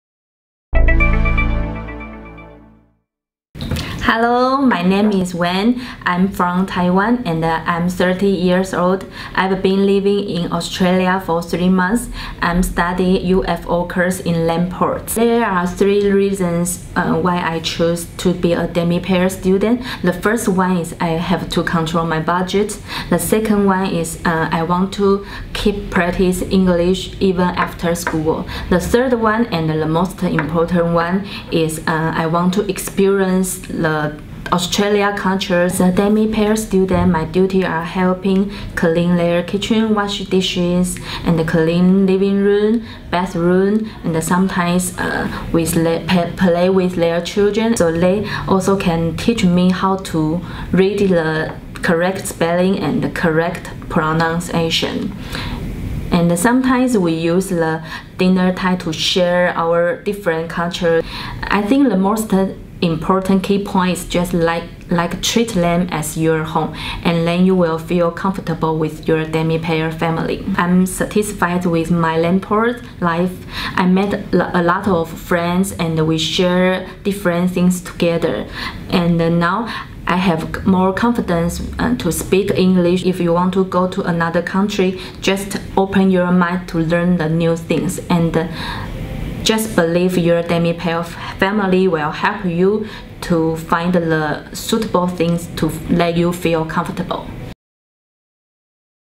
澳洲英語課程+交換換宿 Ezgo 學員Wen心得分享
UFO English + Demi pair Wen from Taiwan
EZGO學員Wen向大家分享他的親身體驗 - 澳洲12週語言課程+交換食宿

What is 【交換食宿English + Demi Program】呢?
利用課餘時間，每周20個小時，協助寄宿家庭家務事處理，與陪伴小朋友(遊戲或做工課)的一個方式 。而寄宿家庭,提供一周 7天, 三餐以及 單人房住宿免費，來當作互換的另類文化體驗。

易格遊學 澳洲遊學/海外遊學專家'